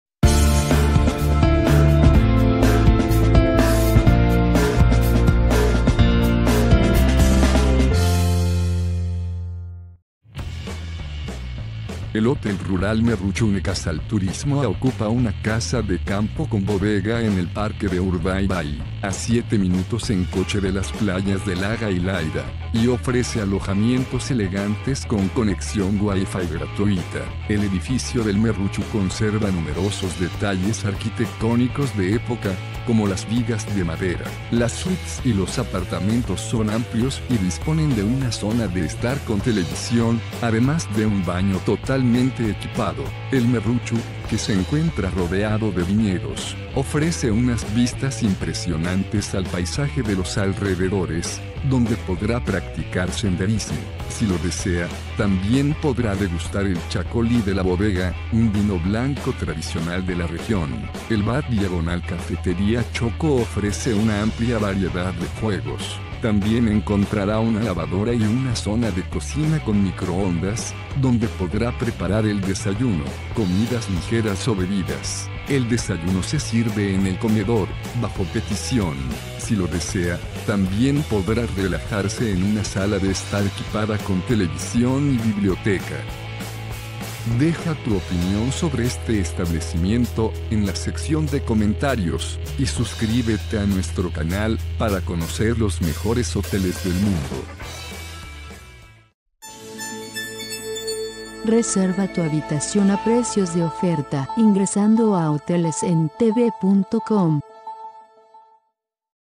H&AP Rural Merrutxu en Ibarrangelu
H&AP Rural Merrutxu es una Casa rural de Ibarrangelu con salas de reuniones / banquetes, jardín y terraza.